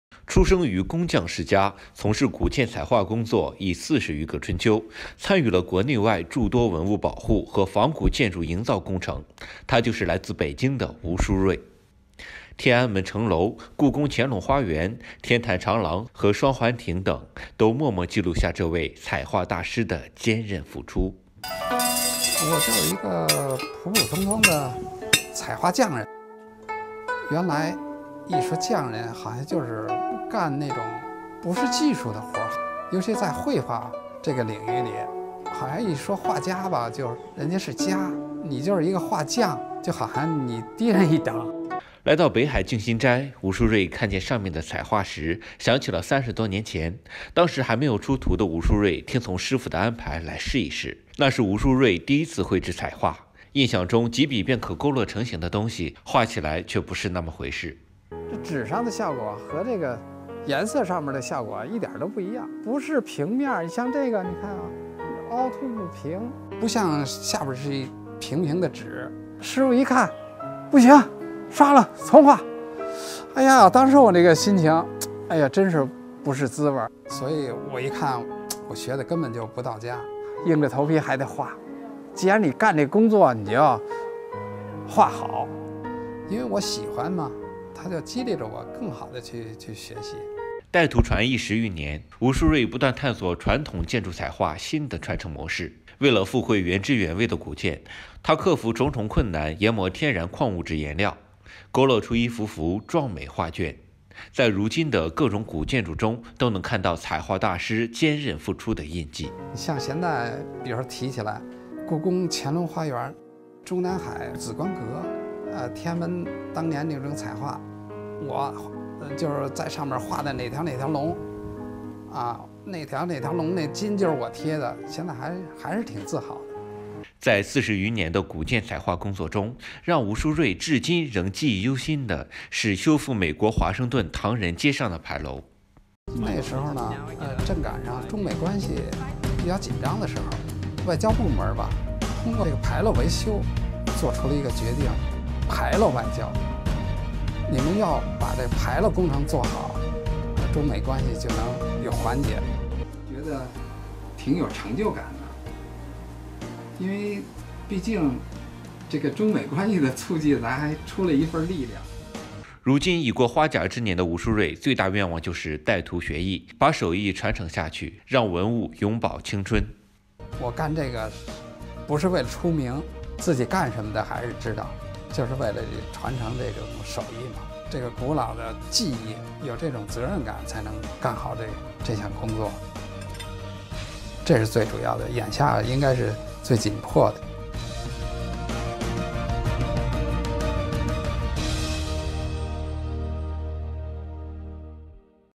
彩画大师吴书瑞 圣手复原壮美古建
吴书瑞出生于工匠世家，从事古建彩画工作已40余个春秋，参与了国内外诸多文物保护和仿古建筑营造工程。天安门城楼、故宫乾隆花园、天坛长廊和双环亭等都默默记录下这位彩画大师的坚韧付出。如今他已过花甲之年，最大愿望就是带徒传艺，把手艺传承下去，让文物永葆青春。